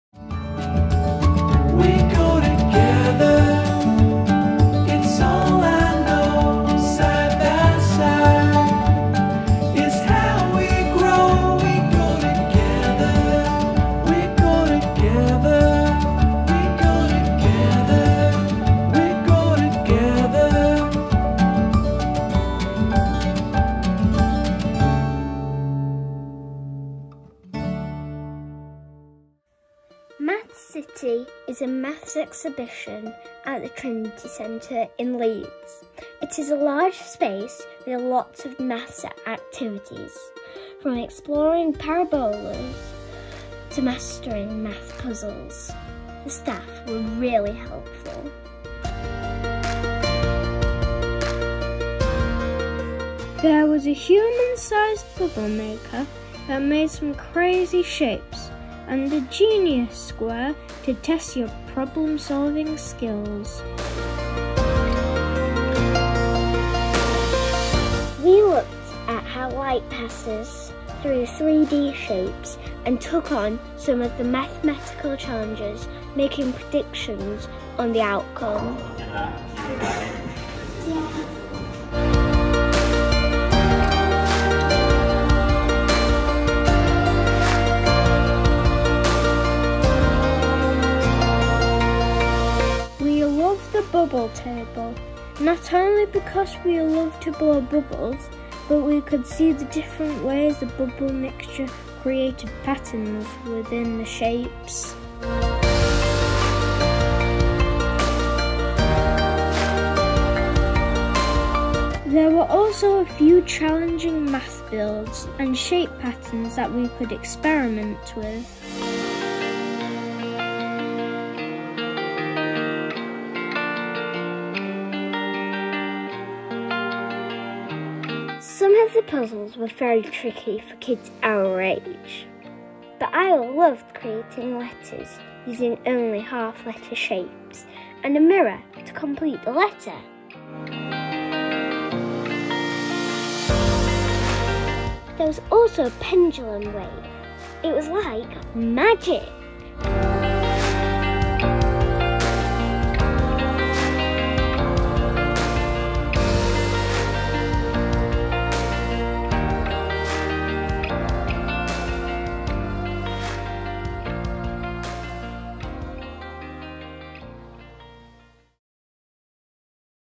Maths City Home Ed visit
We visited Maths City at the Trinity Centre in Leeds for a more hands on maths experience. The staff were really helpful and it made a change to see how math works practically. It really doesn't have to be boring!